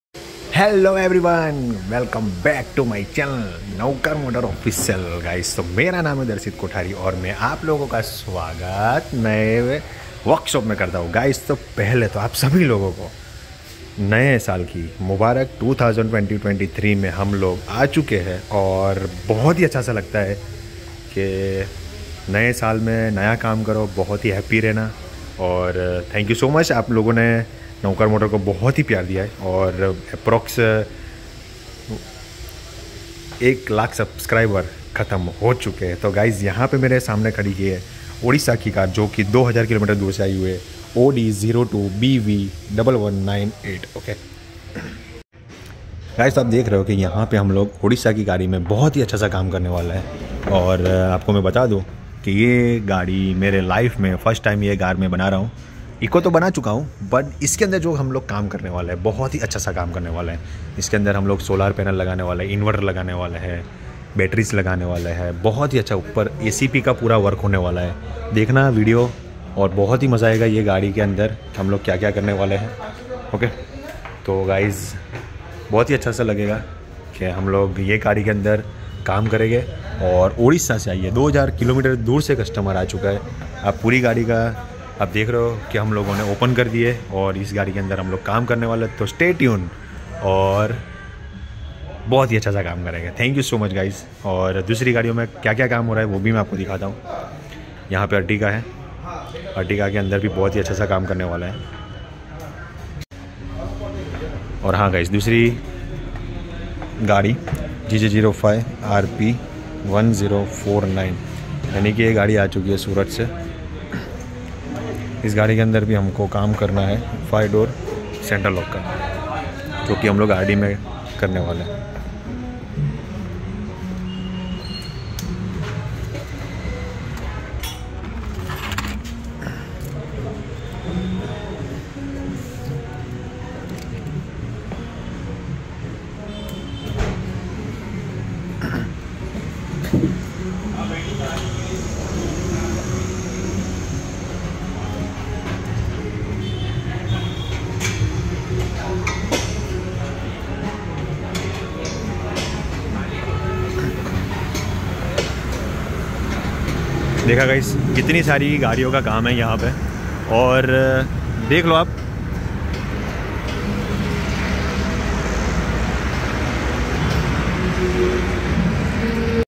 Odisha to Vadodara First time in navkar Motors || Maruti eeco bs6 Modification By Navkar Motors.
Odisha to Vadodara First time in navkar Motors || Maruti eeco bs6 Modification By Navkar Motors || Vadodara || M. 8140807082.

Navkar Motors Official

Vadodara || Gujrat || India